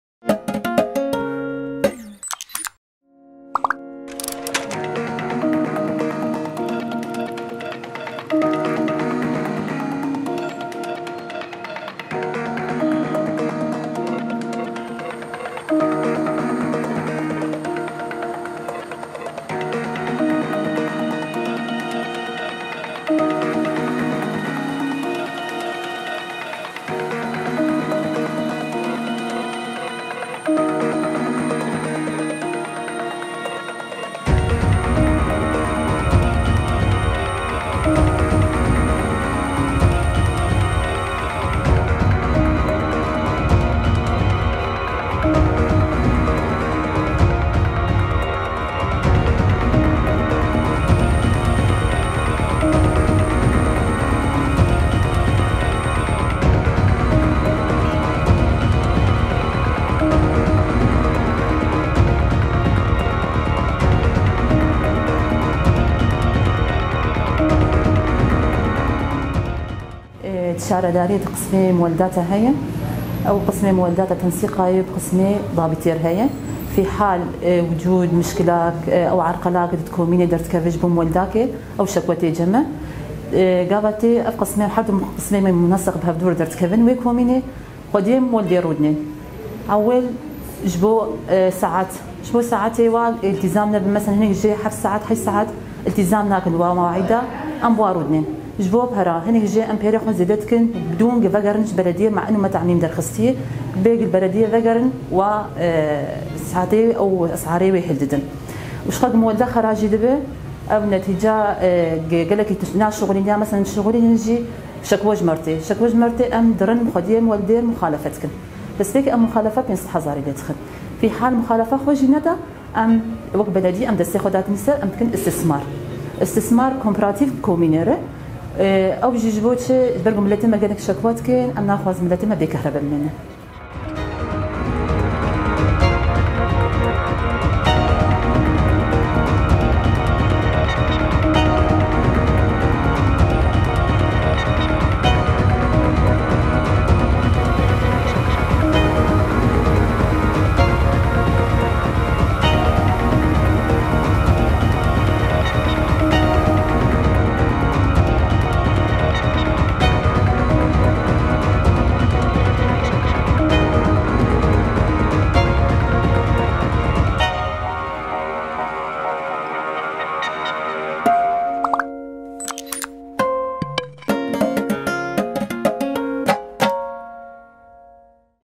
مولدات الكهرباء في القامشلي
فيديو غرافيك|| مولدات الكهرباء في مدينة القامشلي..
كم عددها؟ ما مدى طاقتها الإنتاجية؟ وما هي أسعار الأمبيرات؟